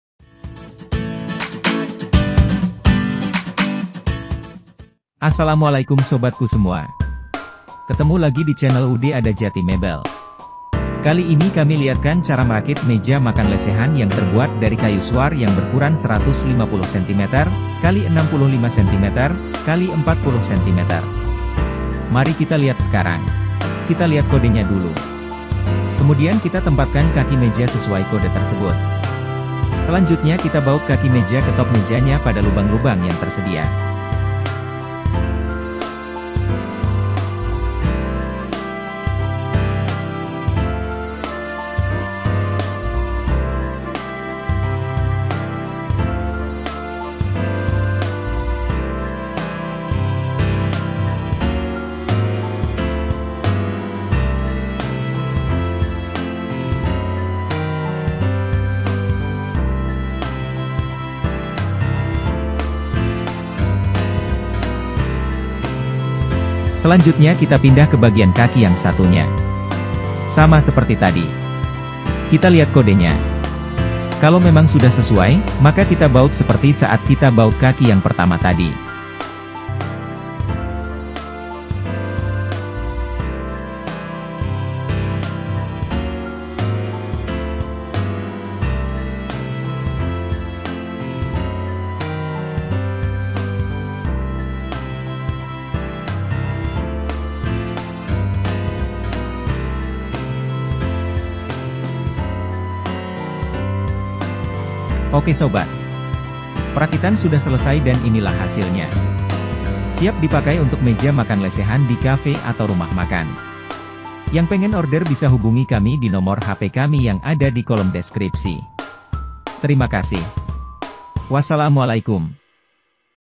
Merakit meja makan lesehan terbuat dari kayu suar atau trembesi
Meja makan yang terbuat dari kayu suar berukuran 150cm x 65cm x 40cm. simple model minimalis. cocok untuk meja makan lesehan di cafe atau rumah makan. yang berninat bisa kontak kami di no hp/wa: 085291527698.